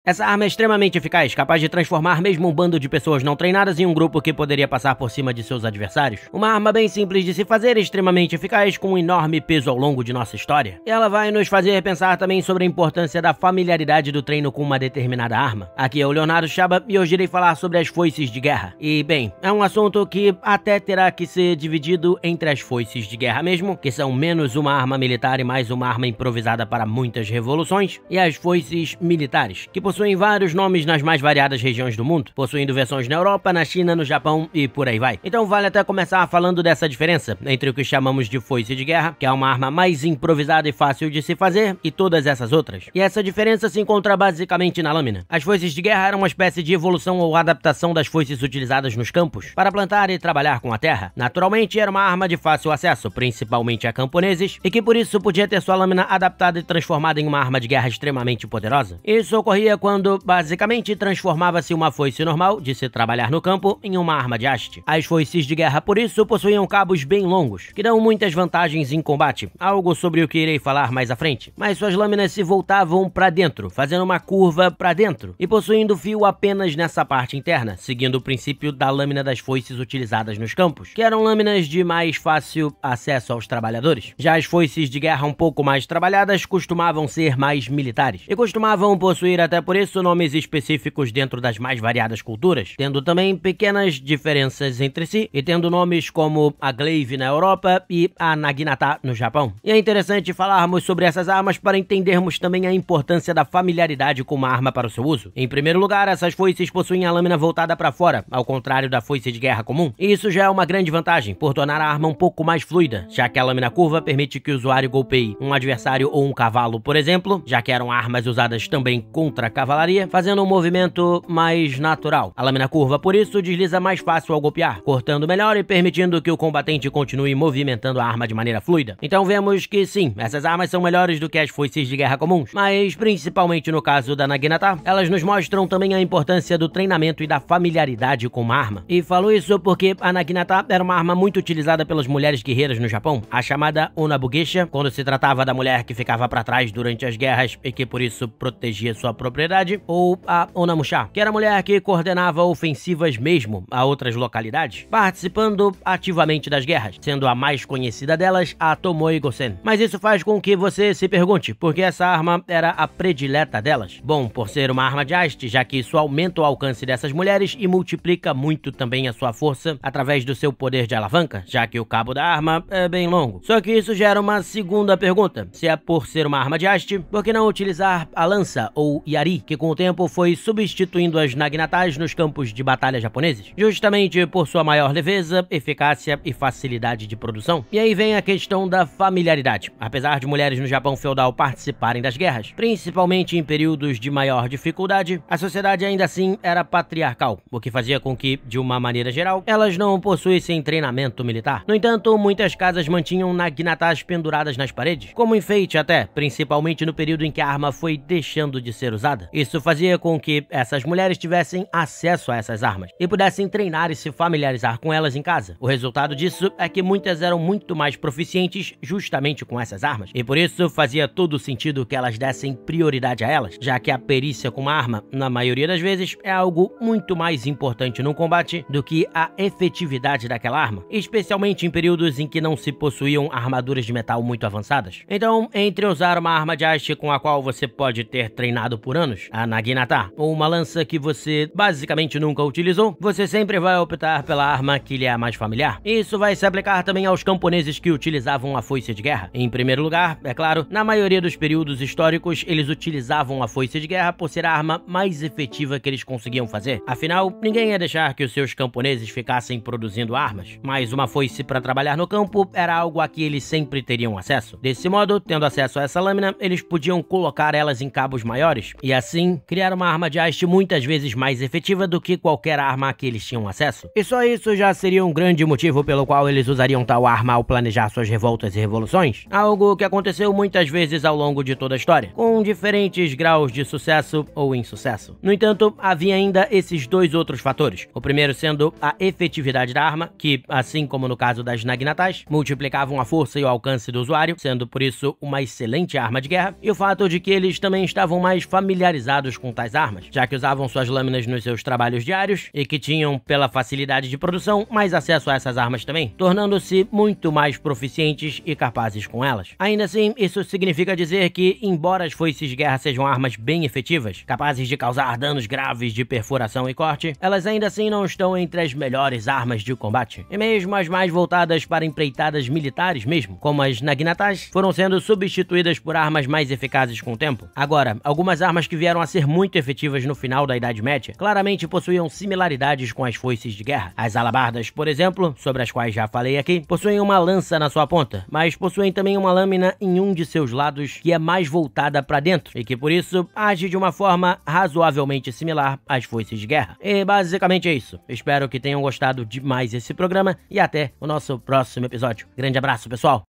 Essa Arma Transformava Camponeses em Guerreiros!
As foices de guerra não são exatamente a arma que todos pensam. São incríveis armas usadas ao longo da história, mas bem pecualiares!

Imagens (via Wikimedia Common) – Wulfgeist, Creator:Hans Stromair, Marshall Astor, Samuraiantiqueworld, Rama, SLIMHANNYA, Fordmadoxfraud, Wolfgang Sauber